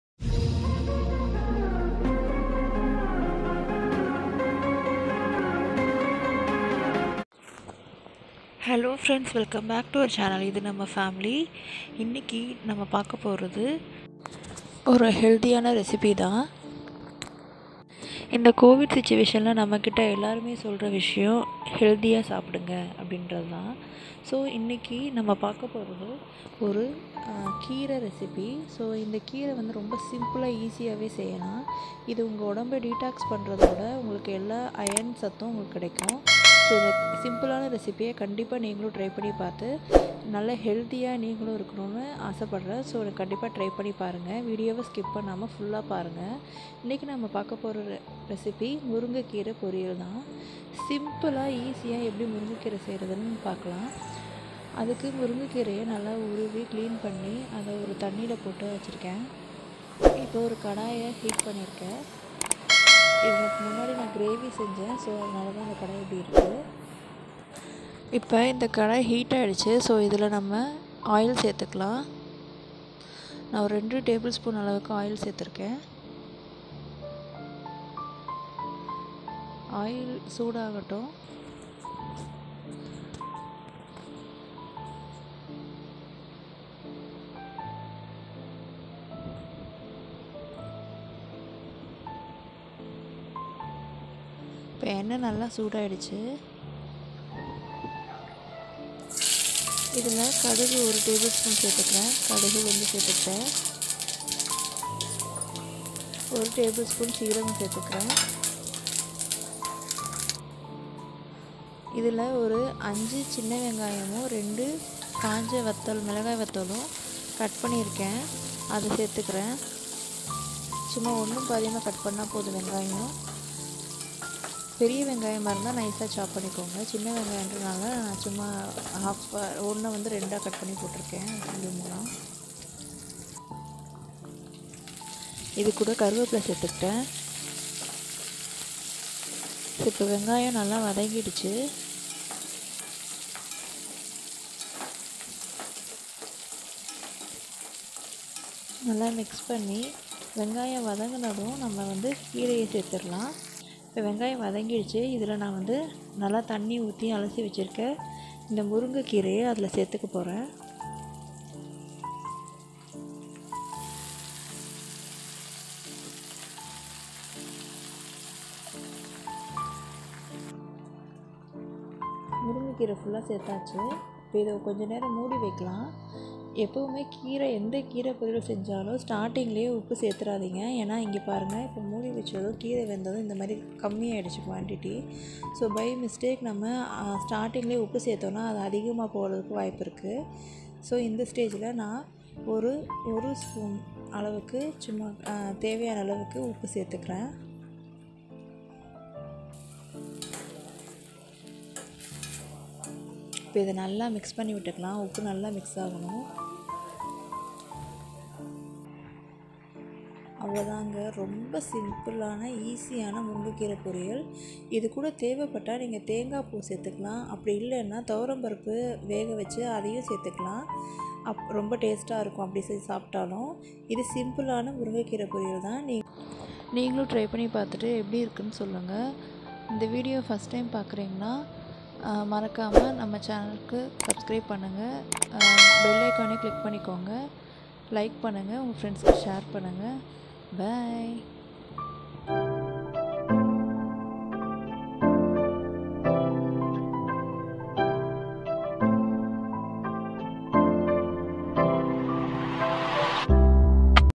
Murunga keerai receipe | Drumstick leaves receipe
In this video we are going to see Murunga keerai poriyal receipe in tamil.

***Ingrediants***
Kadugu ulundhu (Mustard + Urad) - 1 spoon
Cumins (seeragam)-1 spoon
Curry leaves
Salt
Murunga keerai (Drumstic tree leaves)

Channel link : shorturl.at/apvSW

***Subscription is always free***